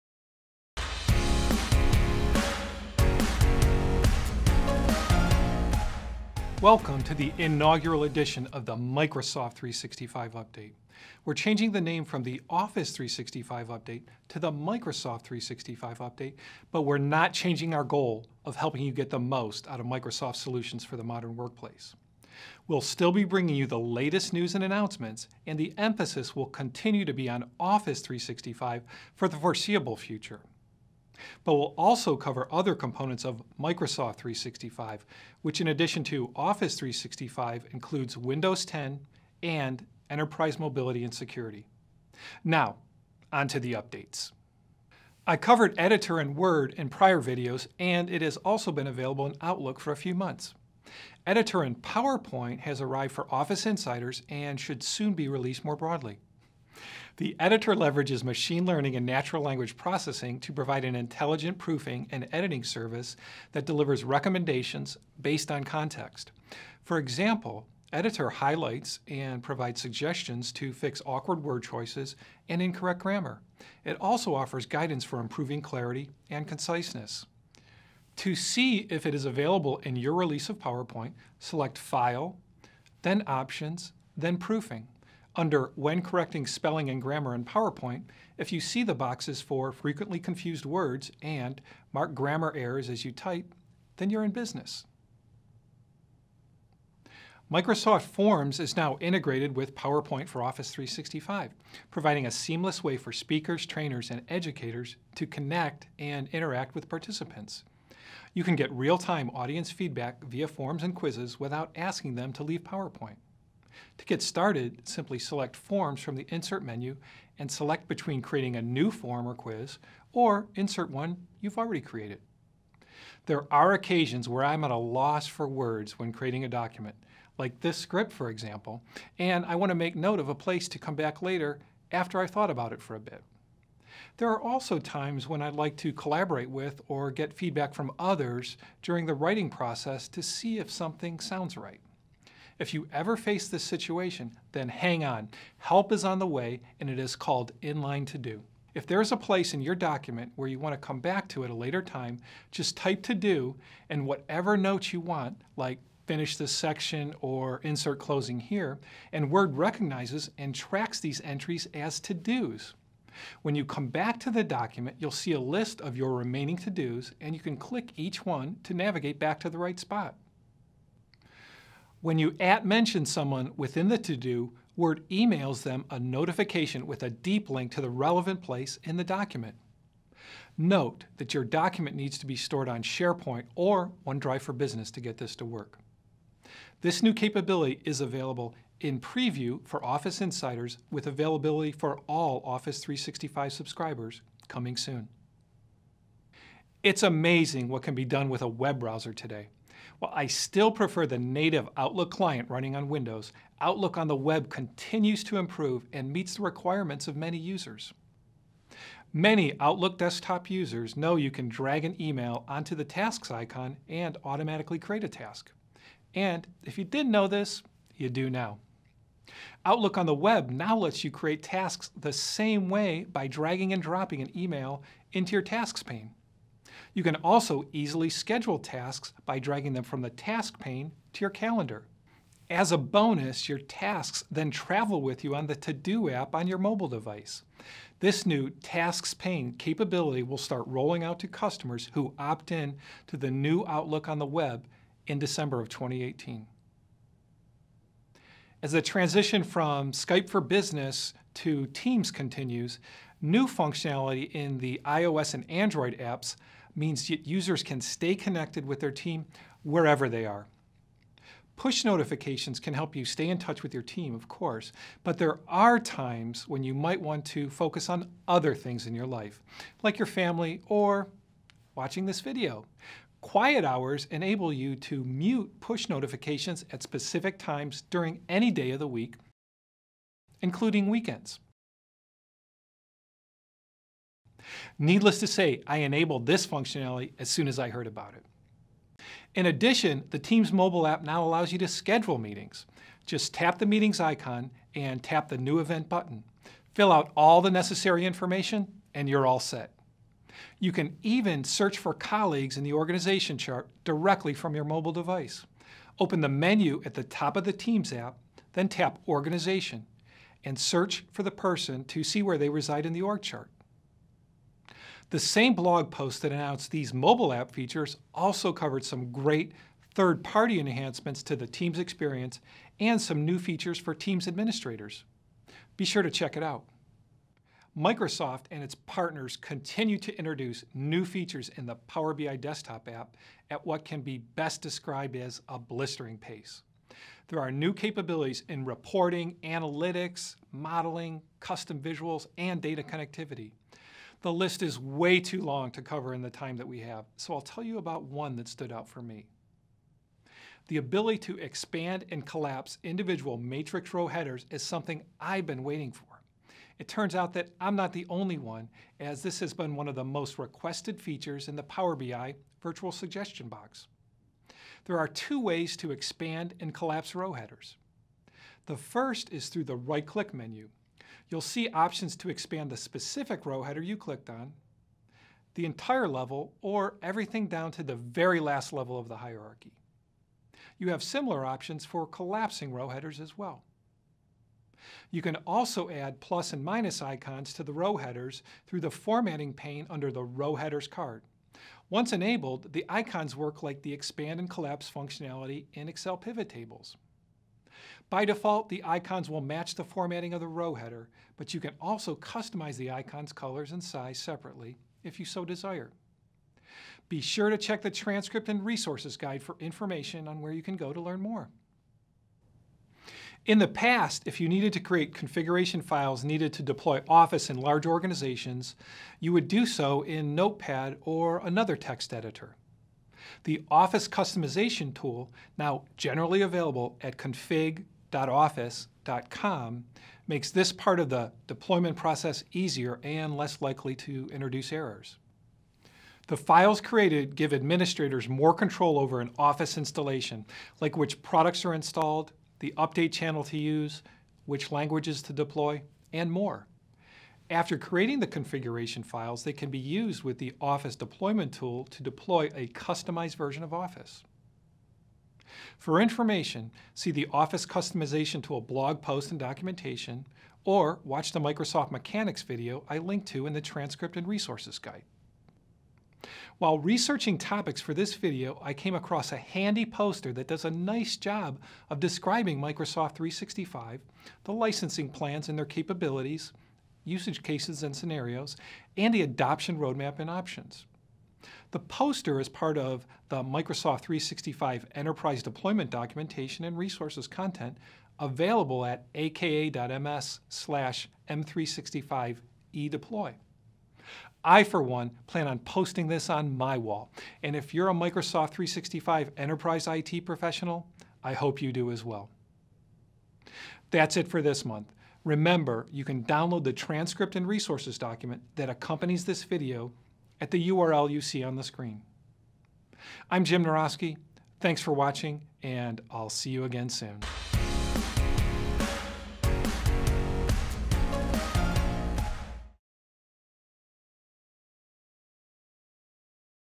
Microsoft 365 update for November 2018
Jim Naroski covers recent enhancements to Microsoft 365 and Office 365, Microsoft's cloud-based productivity solution for the modern workplace. The course transcript, complete with all links mentioned, is available

This month's topics:

    Editor in PowerPoint [0:45]
    Forms in PowerPoint [1:38]
    Word to-dos [2:02]
    Outlook on the web tasks [3:16]
    Microsoft Teams on mobile [4:08]
    Power BI reporting [5:27]
    Office Customization Tool [6:52]
    Microsoft 365 Enterprise poster [7:46]